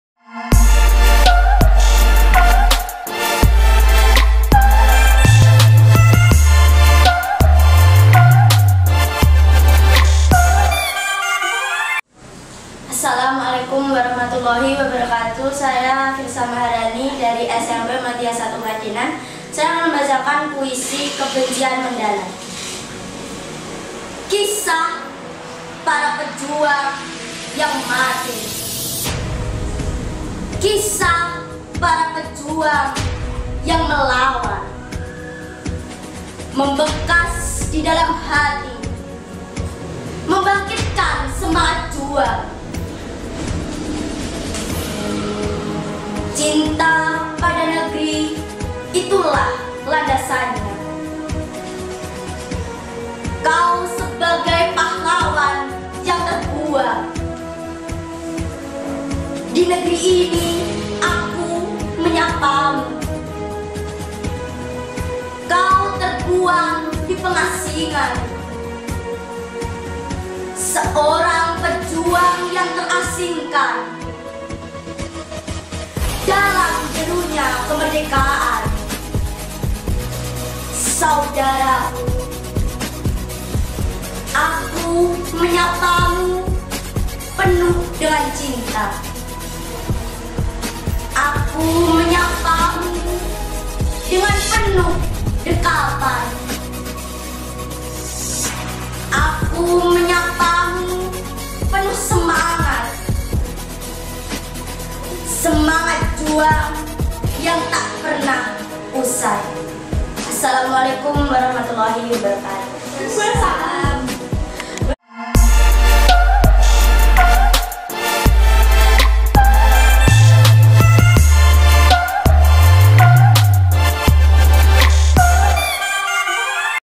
LOMBA BACA PUISI OLEH VIRSA MAHARANI KELAS VIII SMP MUHAMMADIYAH 1 KRADENAN
Selamat menyaksikan video pembacaan puisi oleh anak didik kami atas nama virsa Maharani, sedikit informasi bahwa adik virsa ini, pernah mendapatkan juara 3 lomba baca puisi tingkat kabupaten Blora..